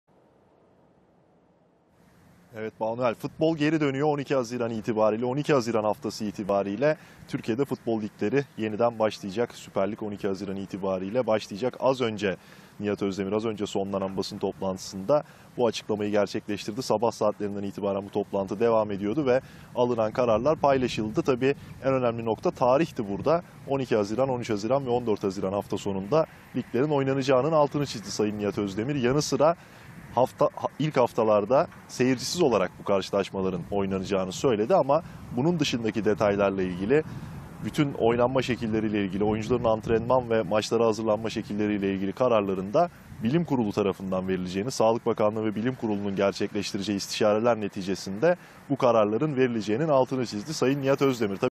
Süper Lig ne zaman başlayacak? TFF Başkanı Nihat Özdemir açıkladı
TFF Başkanı Nihat Özdemir: Yeni tip koronavirüs salgını nedeniyle ara verilen ligleri 12-13-14 Haziran'da başlatma kararı aldık